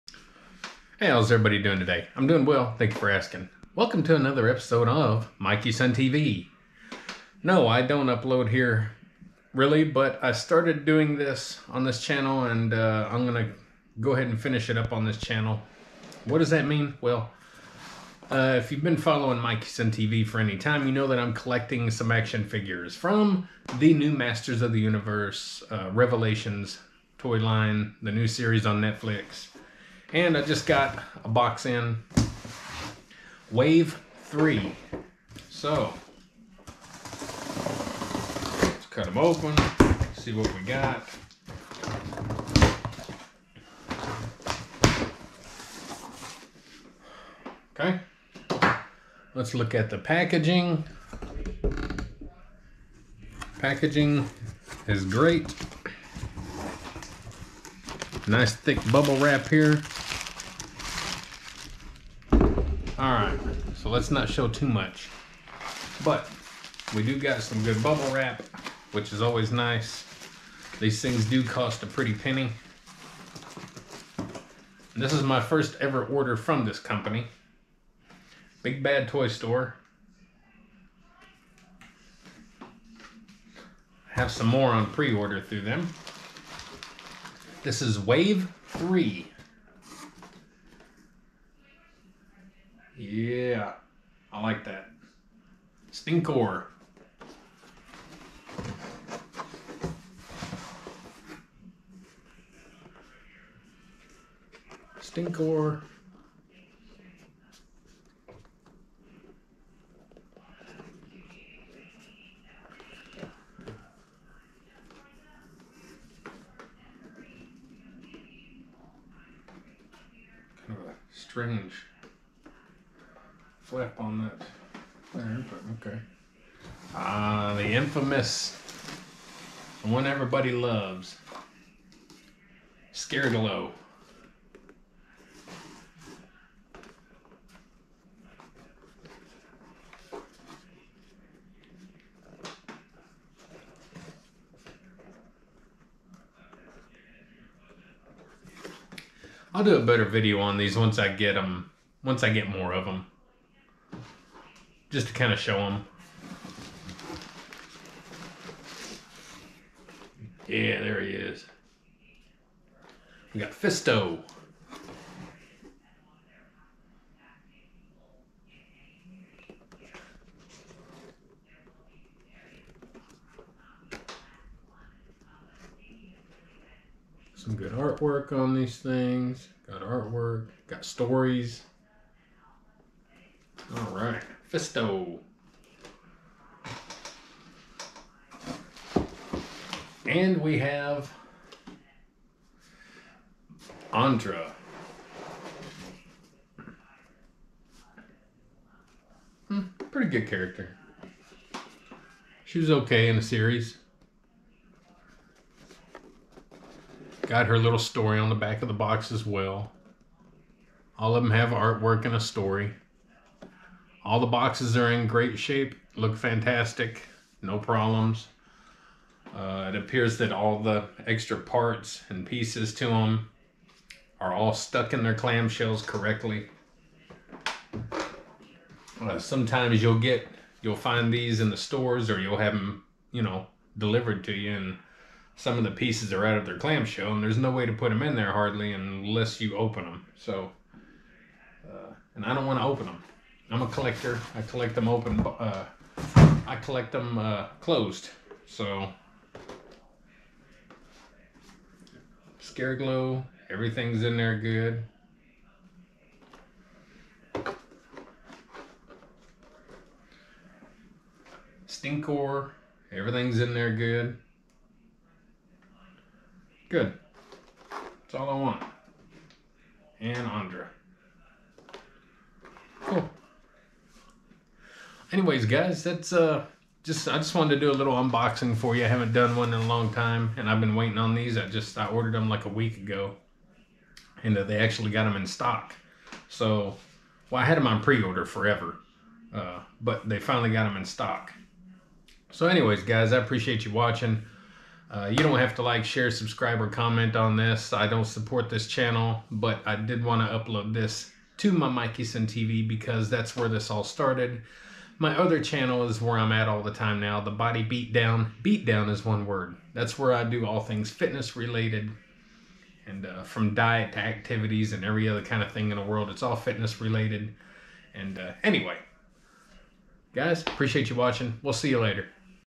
MOTU Revelation Wave 3 BBTS 12 15 21
Finally got my preorder from BBTS.AND a day early.